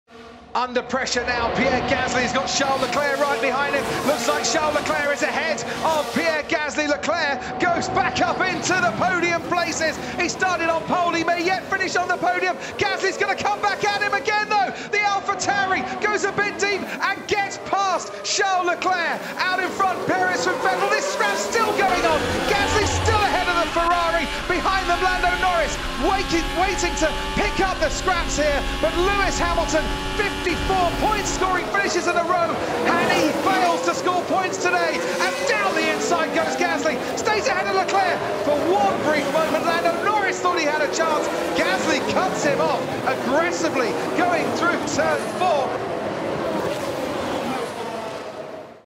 Gasly And Leclerc's Epic Last-Lap Duel in Baku
Pierre and Charles' battle for P3, with Lando Norris also in the mix, had us on the edge of our seats!